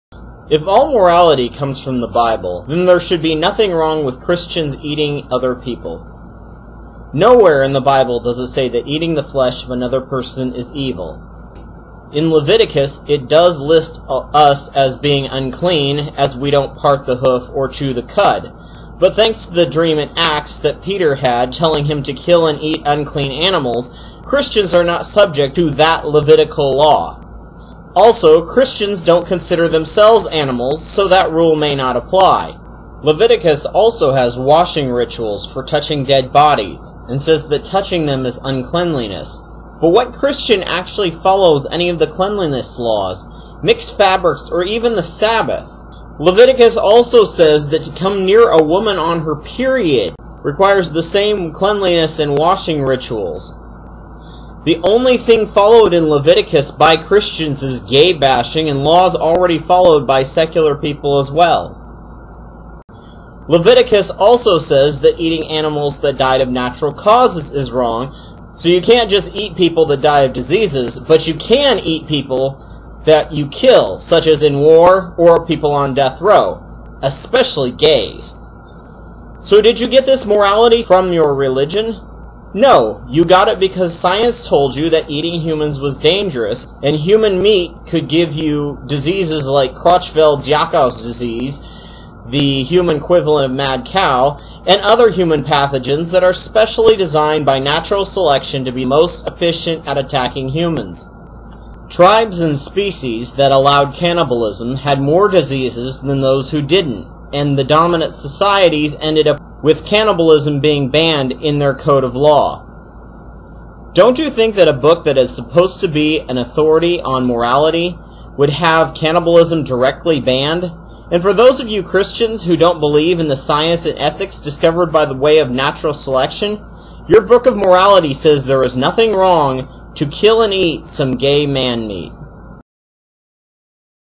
Biblical Morality: Where is Cannibalism?
No where in the book that Christians claim is the source of all morality does the bible say anything banning cannibalism.